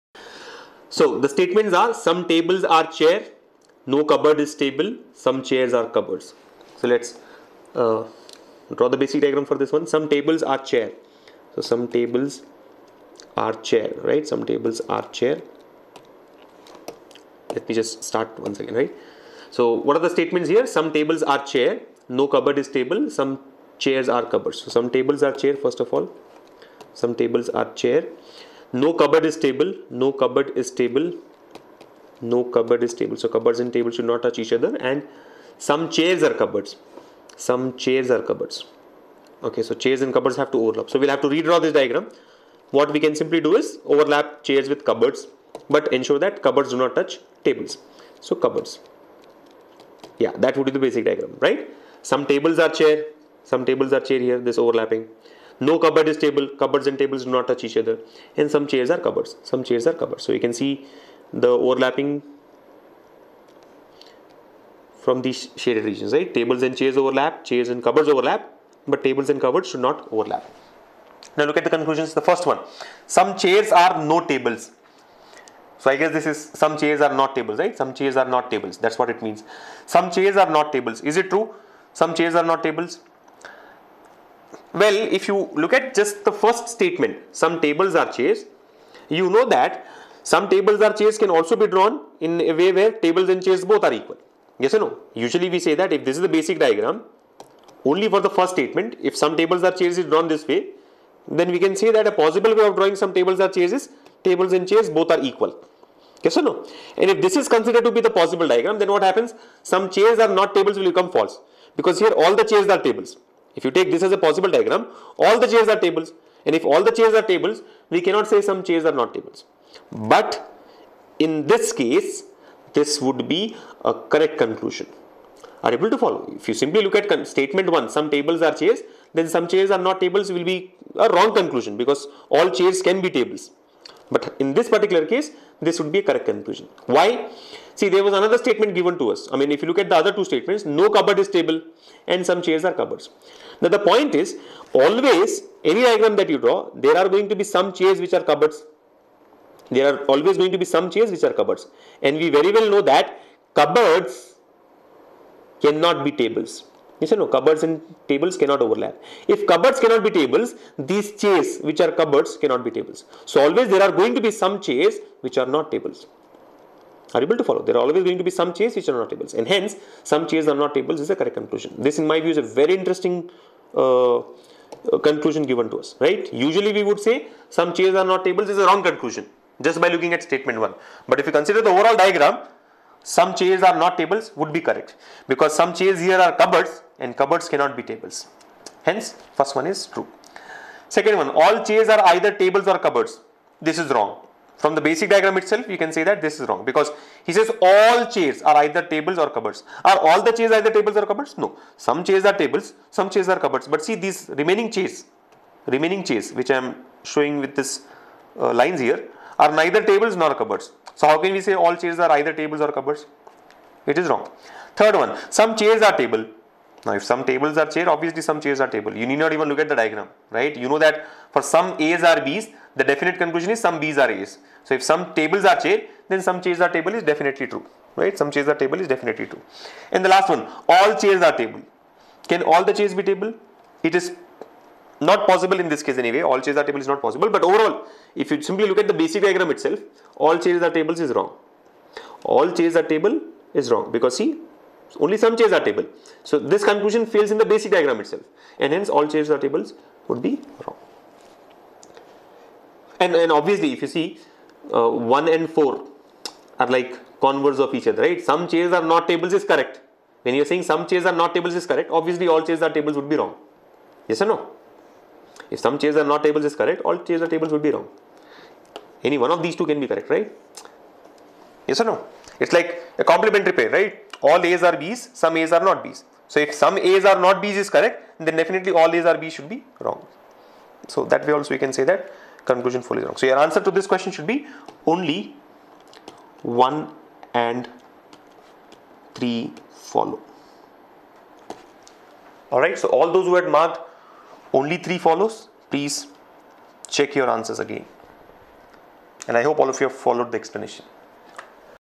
Very Confusing "Some Not Concept" Example of Syllogism| Advanced Example 4 | TalentSprint
Statements:
some tables are chair
no cupboard is table
some chairs are cupboards

Conclusions:
Some chair are no tables
All chairs are either table or cupboards
Some chairs are table
All chairs are table

1) only 1 follows               2) only 2 follows
3) either 3 or 4 follows    4) either 2 and 4 follows
5) None of these

Happy learning!